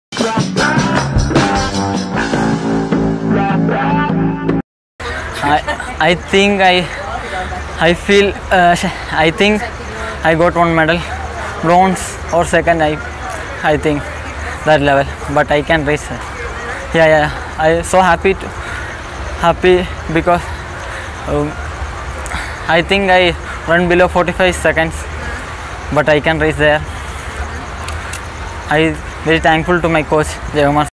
CWG 2018- Muhammed Anas Yahiya finishes fourth at the Men's 400m Final
Muhammed Anas Yahiya fails to win a medal at the 2018 CWG but wins the heart of everyone. He was a bit disappointed after doing too much hard work.